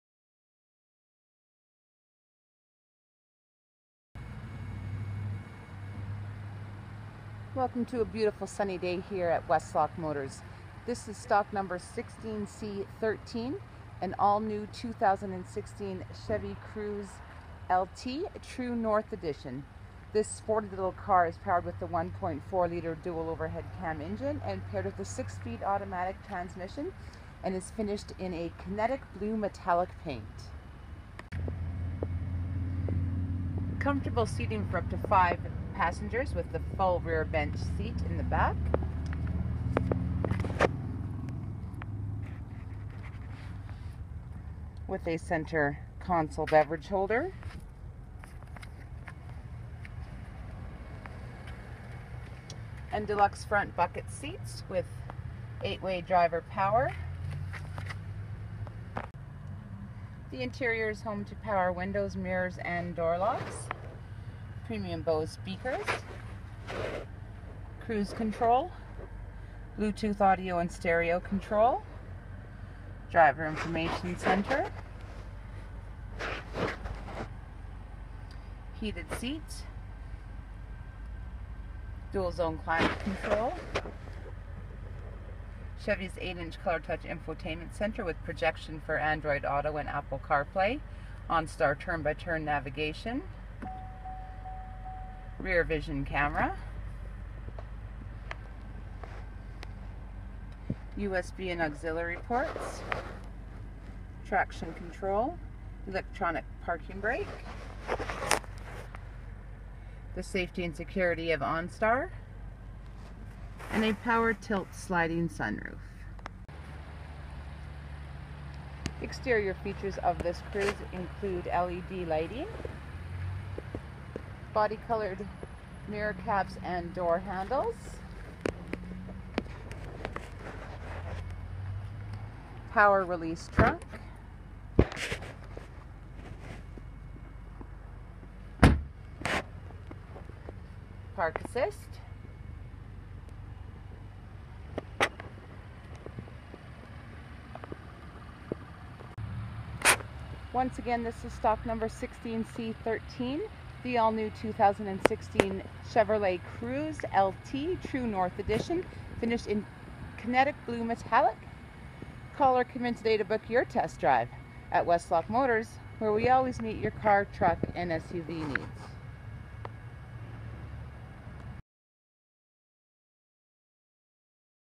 2016 Kenetic Blue Metallic Chevy Cruze LT for sale in Westlock stock # 16C13
Looking for the all new 2016 Chevy Cruze in Edmonton? We have a Kenetic Blue Metallic LT  for sale in Westlock. The all new Cruze is redefined in style and comfort. Powered with the same 1.4L turbocharged engine.The interior is home to heated front seats, power windows mirrors and door locks and a power tilt sliding sunroof. Chevys 8 inch color touch infotainment center with Projection for Android Auto and Apple Car Play. The rear vision camera and rear parking sensors add to the safety and security of this sporty little car. You can effortlessly stream music from your favorite playlist through the premium BOSE sound system. At Westlock Motors, where we always meet your car/truck and SUV needs.